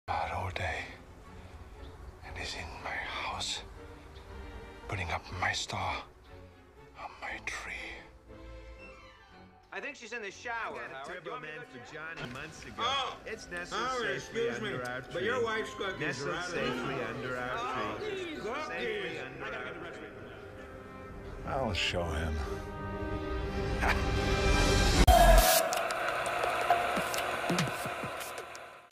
Howard vs. Ted (Round 2 )
Howard and Ted go into the second round with their epic Christmas battle.